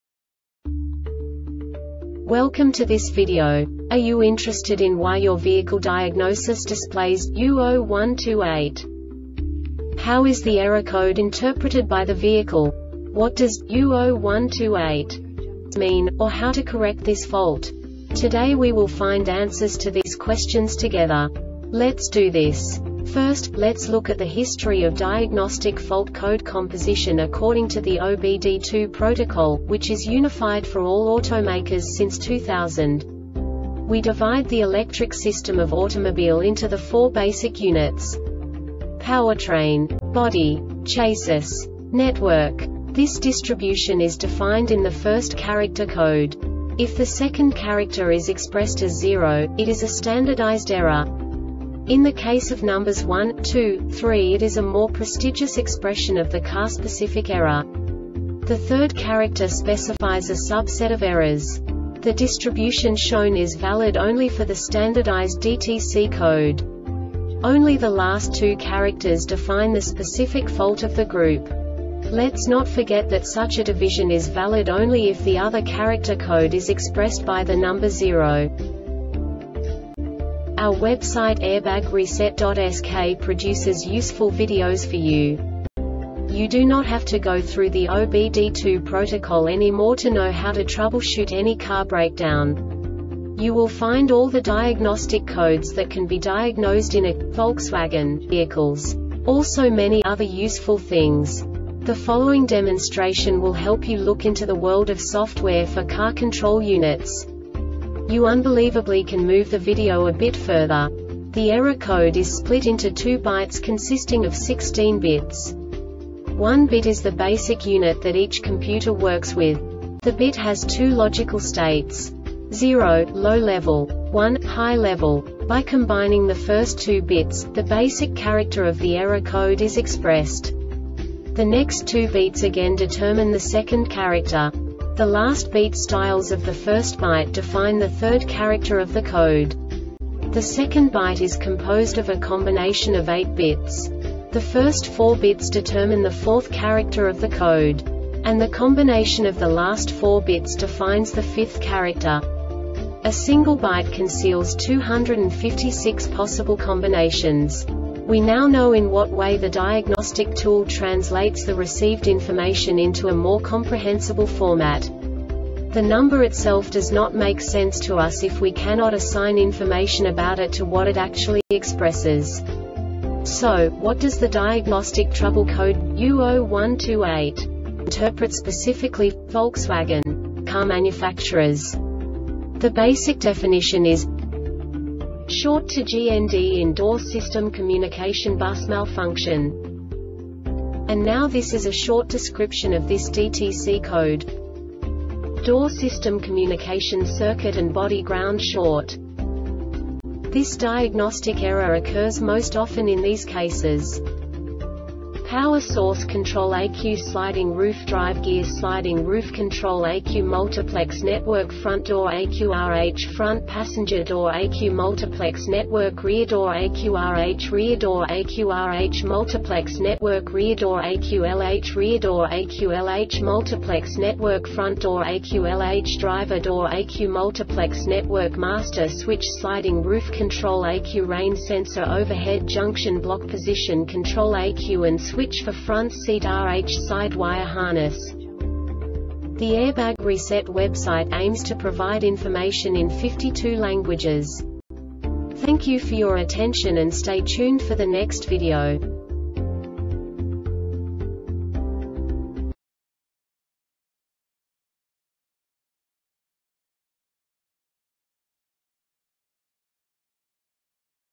DTC Volkswagen U0128 Short Explanation
Contents:
0:21 Basic DTC analysis according to OBD2 protocol standard.
1:48 Insight into programming
2:58 A diagnostically understandable description of the Diagnostic Trouble Code
3:05 Basic error definition

Make:
Volkswagen
Code:
U0128
Definition:
Short to GND in Door System Communication Bus Malfunction
Description:
Door system communication circuit and body ground short
Cause: